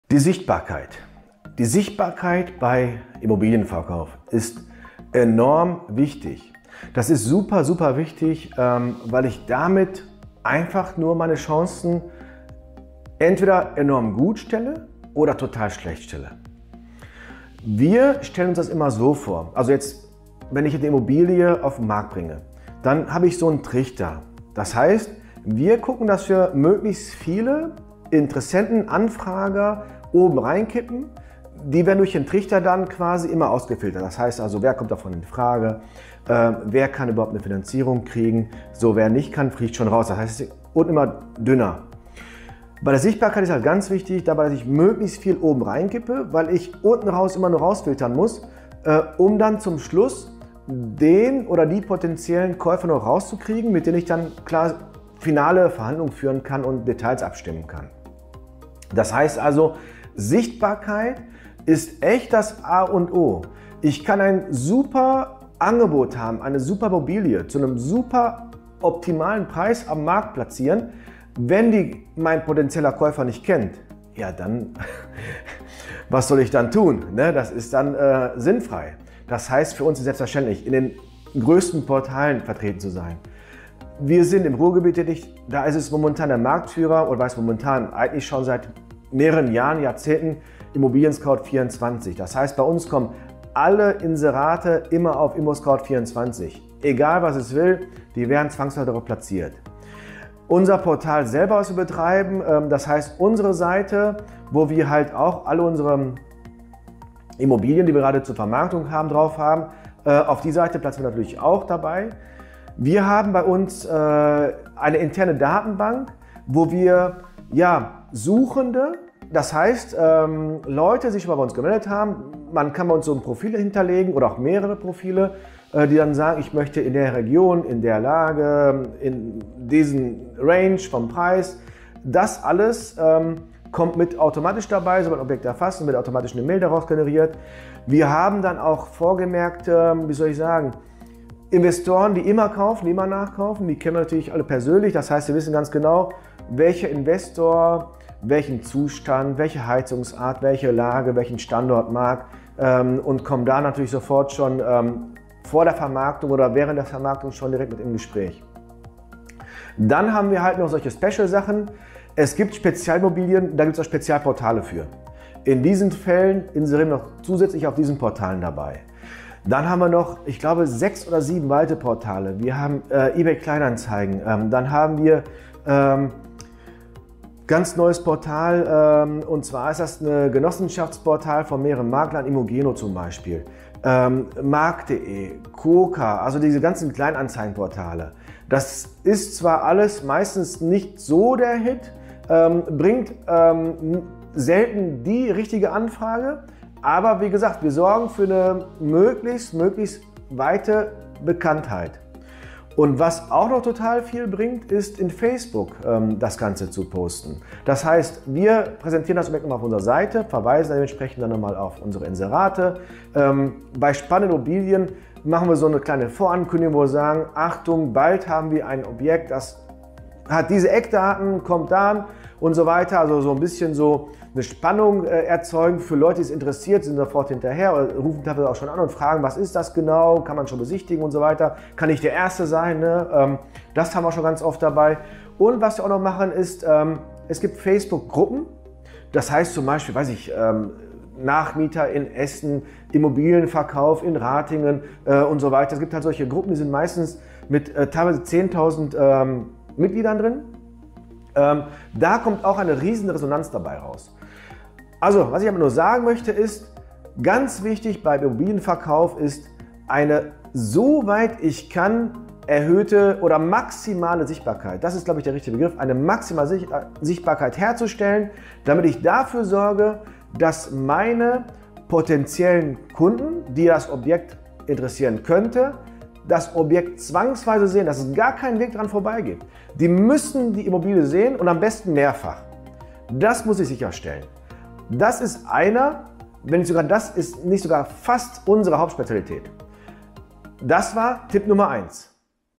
Sichtbarkeit Immobilienverkauf - Immobilienmakler Essen
Beim Verkauf der Immobilie ist vor allem eins Wichtig: die Sichtbarkeit!
Je mehr Interessenten von dem zu verkaufenden Objekt erfahren, um so mehr kaufwillige Kunden kann man finden.
Daher ist eine maximale Sichtbarkeit für einen erfolgreichen Verkauf der Immobilie zum besten Preis sehr wichtig.
Es gilt also die Immobilie auf den wichtigsten Portalen zu veröffentlichen. Das sind in der Regel nicht die kostenlosen, sondern die Marktführer wie ImmoScout24 oder Immowelt/Immonet.

Wichtig ist, wo kann ich den potenziellen Kunden erreichen?
Auch ist es ratsam Aushänge am Objekt anzubringen. Entweder als Plakat von innen an der Scheibe befestigt oder auch Maklernasen.

Oft kommt der Interessent und potenzieller Kunde aus derselben Gegend.

Gute Möglichkeiten sind auch Aushänge am schwarzen Brett bei nahe gelegenen Geschäften.
Vor allem ältere Menschen prüfen die Aushänge genau.

Es lohnt sich auch in speziellen Facebook-Gruppen und auf dem Facebook eigenen Anzeigenmarkt zu posten. Passende Gruppen wie z. B. Immobilienverkauf Essen oder Immobilienverkauf Ruhrgebiet  haben deutlich über 10.000 Mitglieder.
Auch gibt es Stadtteil spezifische Gruppen wie z. B. Die Rüttenscheider oder Essen Holsterhausen, in denen auch die Anzeige gepostet werden kann, wenn sich das Objekt in dem Stadtteil befindet. Hier hat man direkt den richtigen Bezug zum Stadtteil.